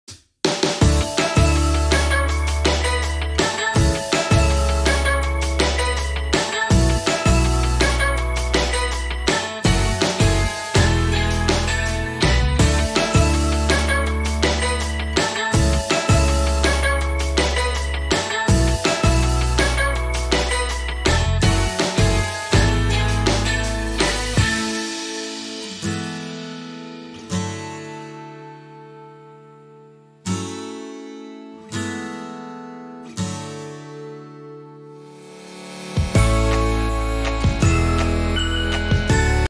Center mall parking lot time lapse with Go Pro Camera
The GoPro time-lapse mode takes photos at preset intervals to stitch into a video. The result is a video where it appears as if time has sped up.